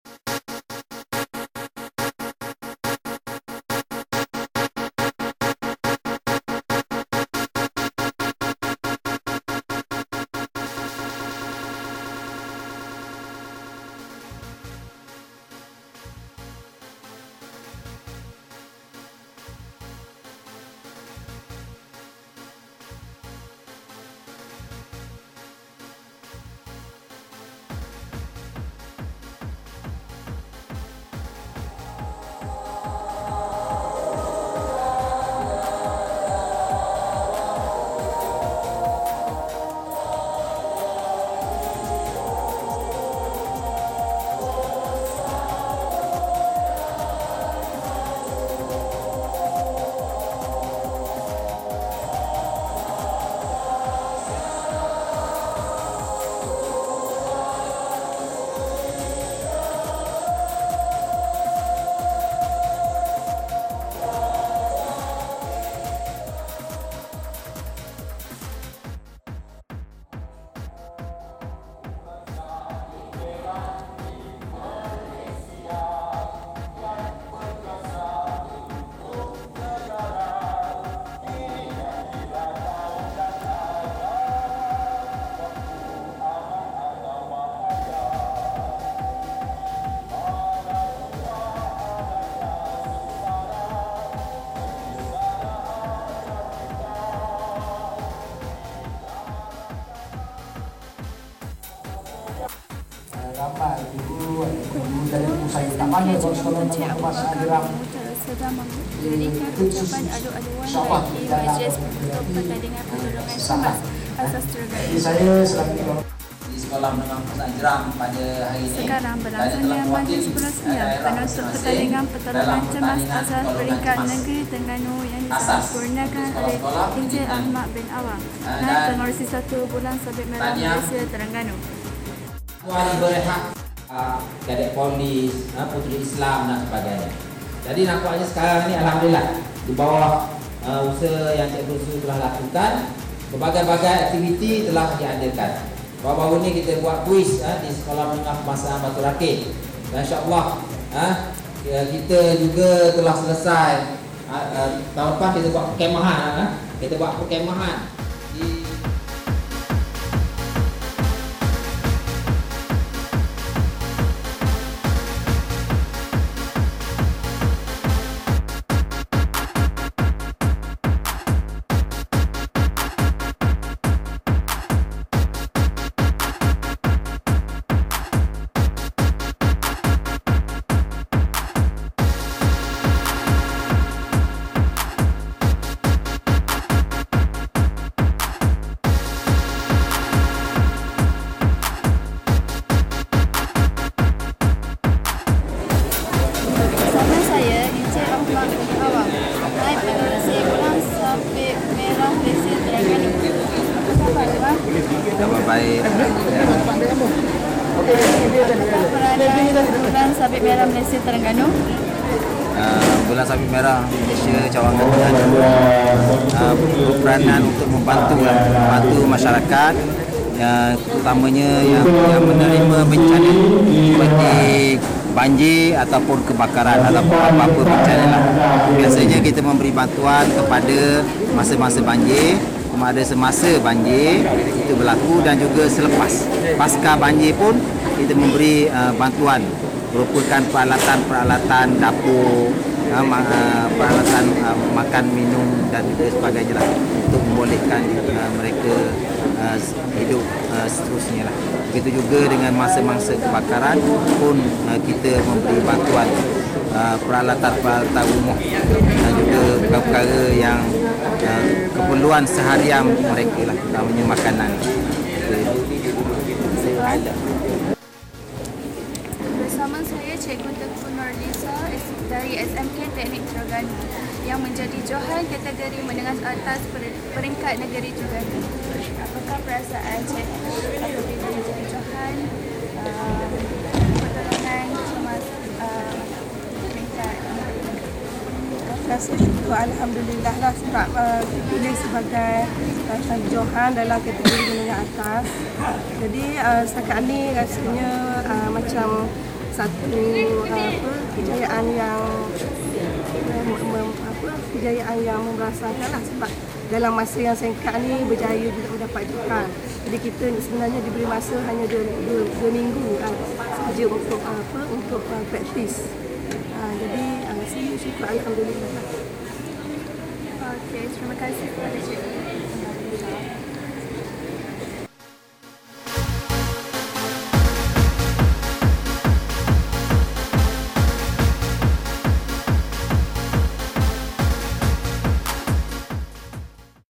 TVJER:EP.86:PERASMIAN PERTANDINGAN PERTOLONGAN CEMAS ASAS NEGERI TERENGGANU 2016
EP.86: MAJLIS PERASMIAN PERTANDINGAN PERTOLONGAN CEMAS ASAS NEGERI TERENGGANU YANG DISEMPURNAKAN OLEH EN.AHMAD BIN AWANG, NAIB PENGERUSI 1 BULAN SABIT MERAH MALAYSIA TERENGGANU
19 OKTOBER 2016